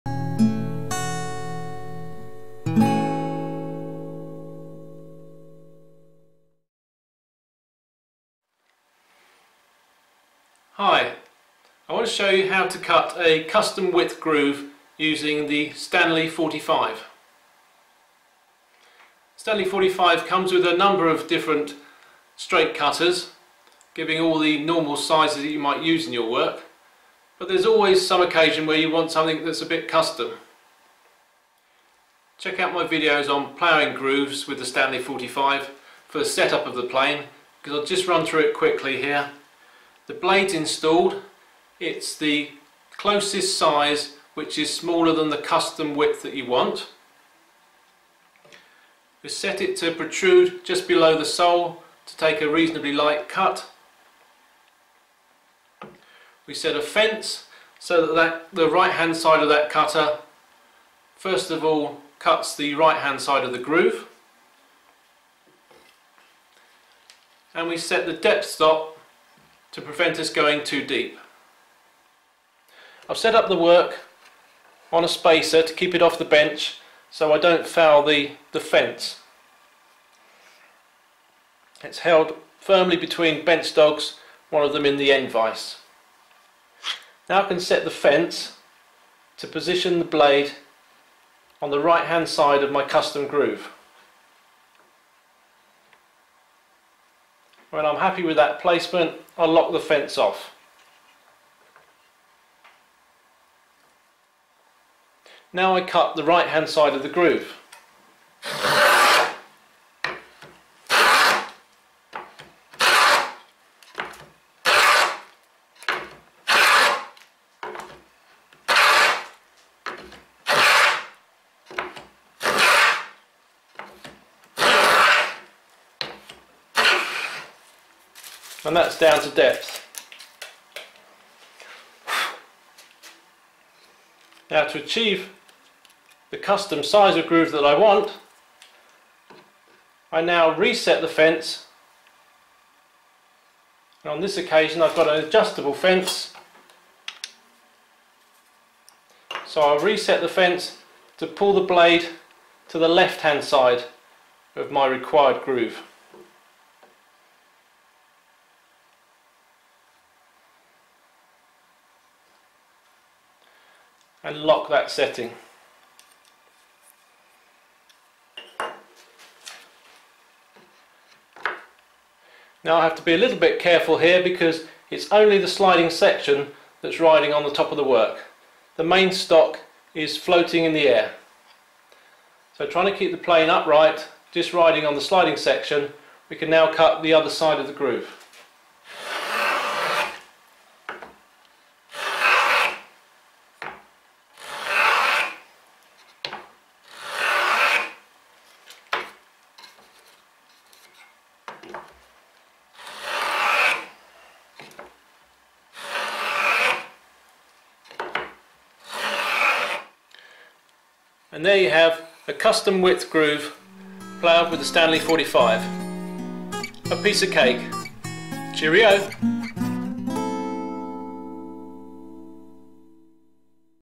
Plough Custom Width Grooves using a Stanley No.45
Ploughing (plowing) non-standard width grooves using the Stanley No.45 is easy once you know how. Following on from my previous video, I'll demonstrate how, all in just a few minutes.

Etsy shop 'Tree2Gift'